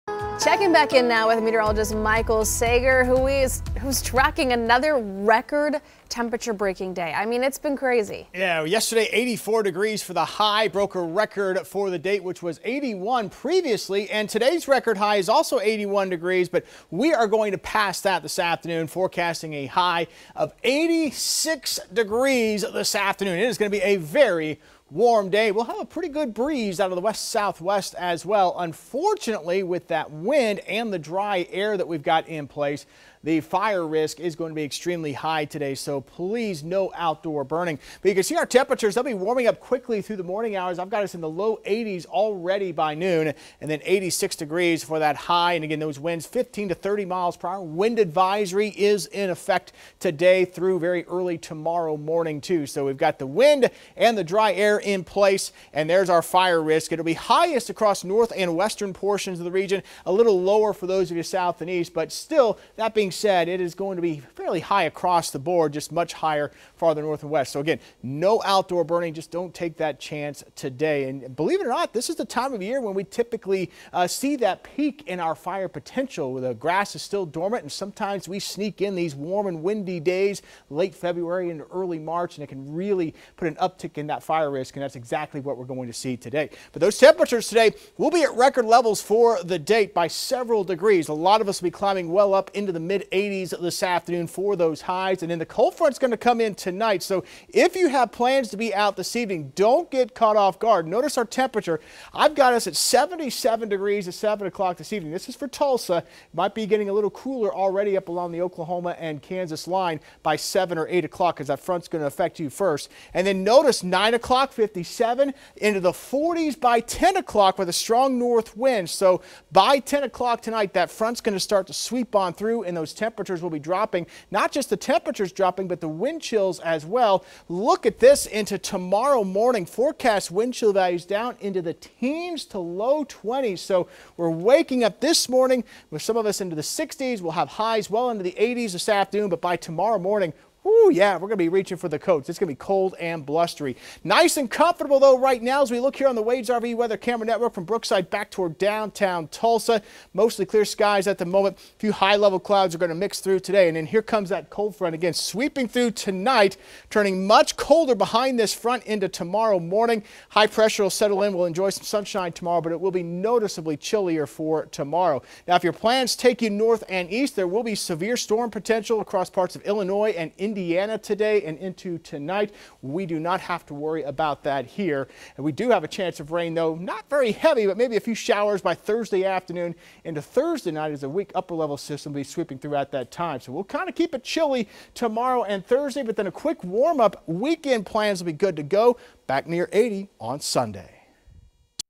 Extremely high fire danger for Tuesday
Record warmth likely this afternoon. Strong cold front arrives tonight with a significant drop in temps into Wednesday morning.